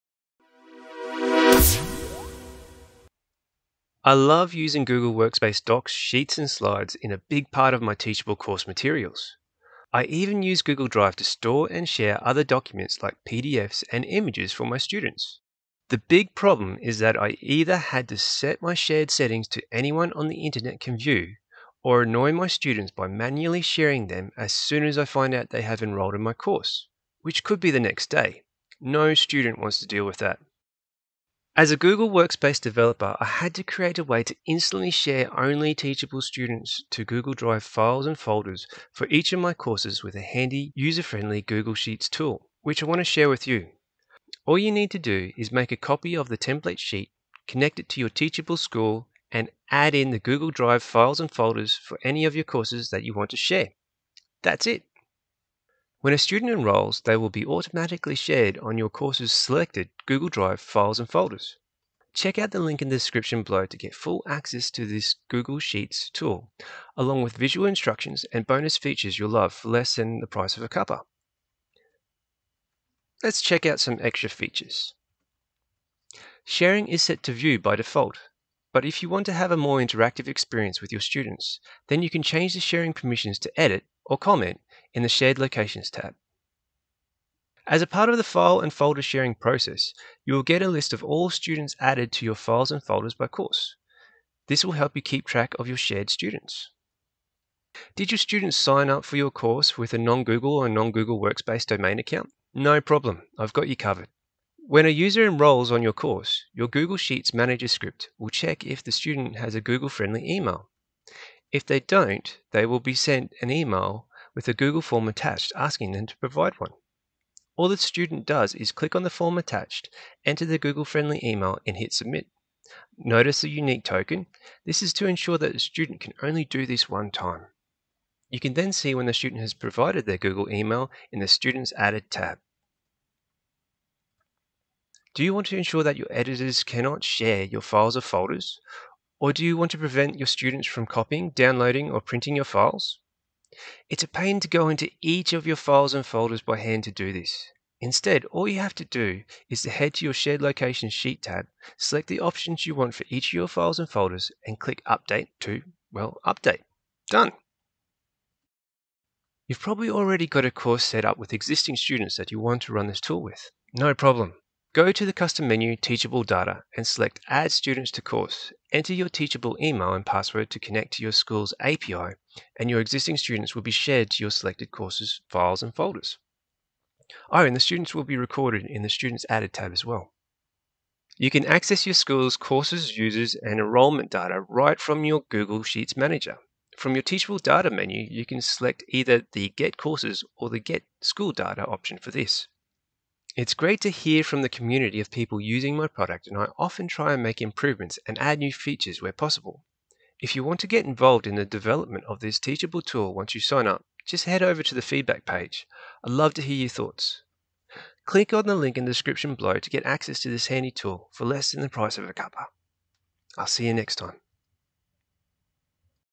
Share ONLY Teachable students to Google Drive files and folders when they enroll
Grab this awesome tool here!

You don't want to give the whole world access to your Teachable course materials.
Find out how to instantly share ONLY Teachable students to selected files and folders in Google Drive when they enrol.

00:00 About the Tool
01:15 Sharing Options
01:28 List of students added by course
01:38 Dealing with non-Gmail & non-Google Workspace Domain emails
02:13 Granular Sharing Permissions
02:40 Share Existing Students
03:02 Courses, Users & Enrollment Data
03:14 Get Involved!